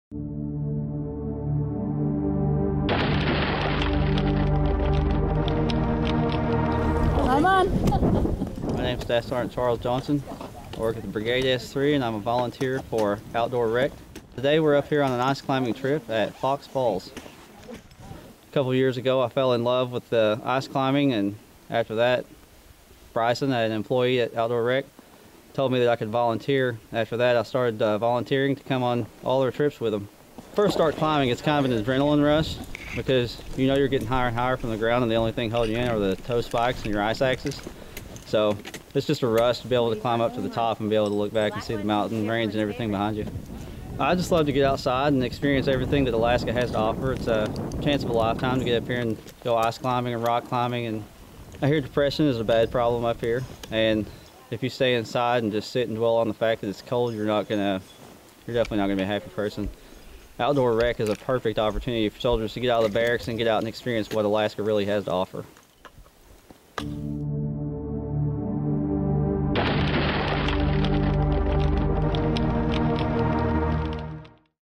SSG Charles Johnson, Ice Climber
1st Stryker Brigade Combat Team's SSG Charles Johnson takes us ice climbing at Fox Falls, near Fox, Alaska, and shows us the ropes.